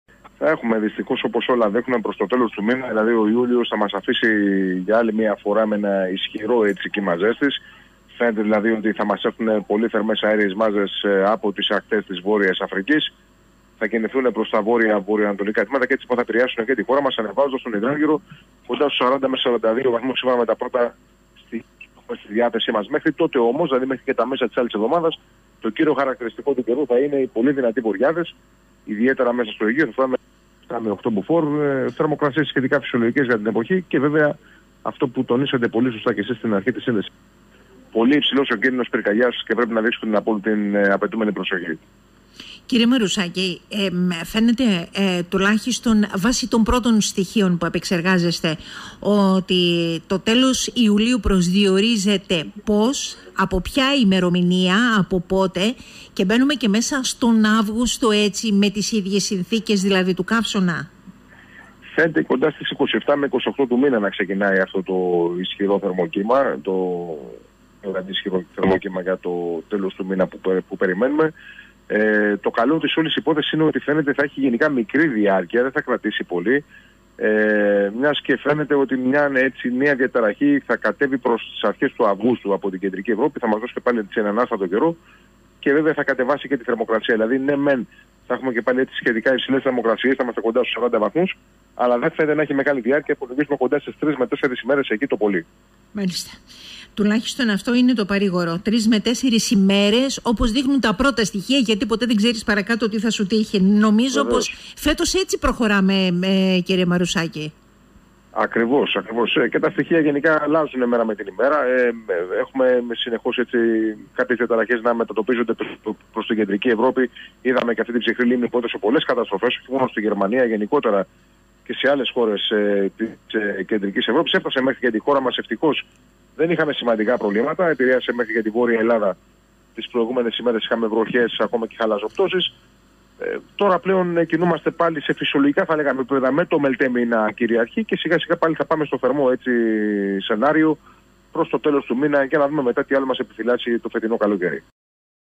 Κλ. Μαρουσάκης: Επιστρέφει ο καύσωνας τέλος Ιουλίου
Ευτυχώς θα είναι μικρής διάρκειας

Οι θερμές αέριες μάζες από τις ακτές της βόρειας Αφρικής επιστρέφουν στη χώρα, με τον υδράργυρο στο τέλος Ιουλίου, από τις 27 και μετά, να φθάνει στους 40 και 42 βαθμούς κελσίου.

Ευτυχώς όμως η διάρκεια παραμονής στη χώρα μας θα είναι μικρή όπως μας εξηγεί ο μετεωρολόγος Κλέαρχος Μαρουσάκης, καθώς στη χώρα θα φθάσει νέο ψυχρό μέτωπο από την Ευρώπη.

Προς το παρόν ωστόσο, το ενισχυμένο μελτέμι χαρίζει ανάσες δροσιάς, αν και ο κίνδυνος πρόκλησης πυρκαγιάς σε πολλές περιοχές βρίσκεται στην κατηγορία 4.

Σε επιφυλακή βρίσκετε το Πυροσβεστικό Σώμα, καθώς σύμφωνα με το Χάρτη Πρόβλεψης Κινδύνου Πυρκαγιάς, που εκδίδει η Γενική Γραμματεία Πολιτικής Προστασίας, για σήμερα Πέμπτη 22 Ιουλίου 2021, ο κίνδυνος πυρκαγιάς είναι πολύ υψηλός σε:  Χίο, Σάμο, Ικαρίας,  Ρόδο,  Κάρπαθο, Κω, Κάλυμνο.